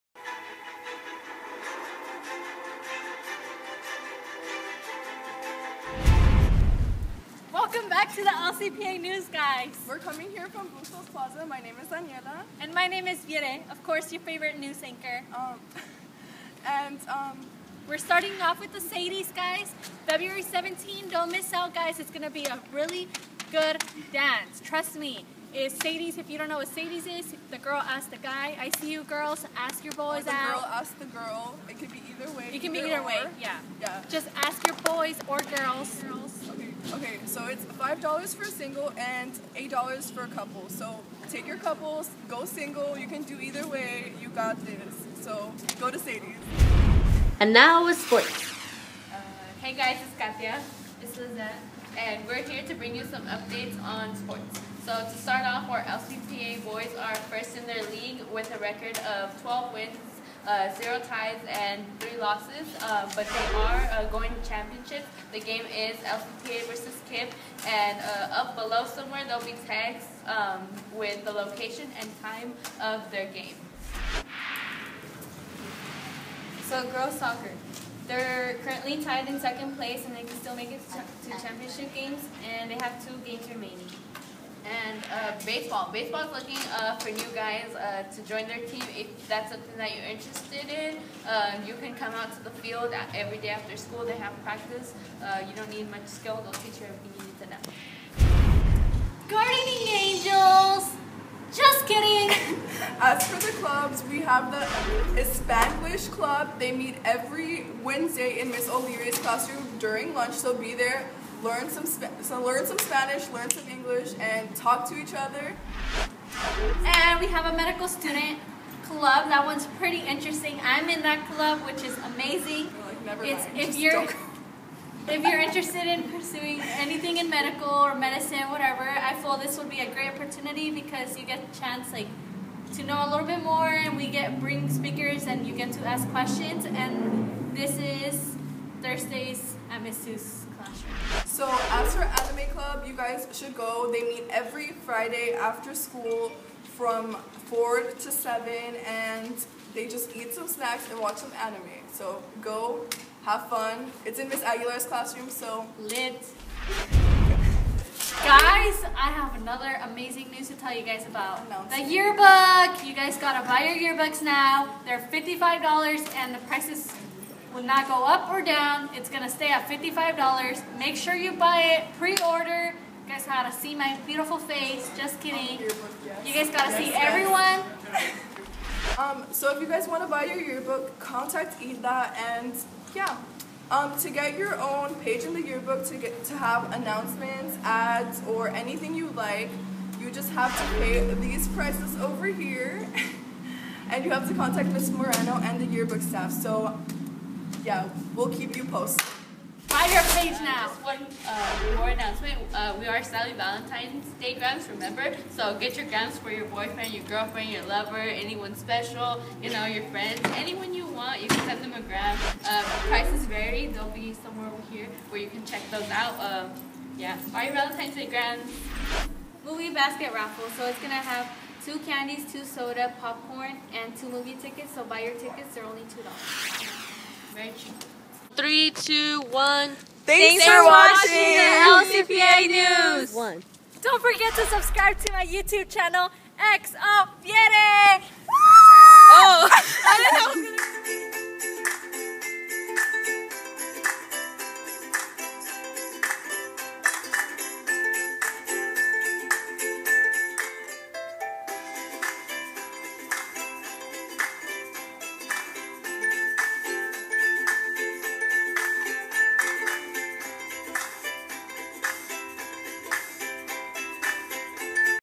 LCPA NEW 2 6 17
Boletos de Sadies
$5 individual
$8 en pareja

Ventas del Dia de San Valentin
Bolsa de dulces $3
Rosas de chocolate $4
Pastelito $5

Rifa de Canasta de Pelicula
$2 por boleto

Ordenen sus anuarios! $55 con Hilda en el segundo piso
Para comprar su propio pagina, los precios son los siguientes:

1/4 pagina : 5$
1/2 pagina: 10$
Pagina completa: 20$

Reuniones de Clubs

Espanglish
Todos los Miercoles en el almuerzo donde O'Leary

Asociacion de Estudiantes de Medicina
Durante Almuerzo los Jueves en Suess

Anime
Los Viernes despues de escuela con Aguilar

Deportes

Futbol
Estan primeros en la liga, y en lugar 24 de CCS.
Juego de Campeonato sera en Independence el dia Miercoles 2/15 @ 7 PM (LCPA V KIPP)

Futbol Lady Eagles
Empatados para segundo lugar, con oportunidad de ir al Campeonato. Les faltan dos partidos.

Beisbol
Estan aceptando nuevos jugadores. Todos los Dias despues de escuela hay practica.

Para poner anuncios o salir en las noticias, ponganse en contacto con Silvia Nolasco.

Subscriben a XOVIERE .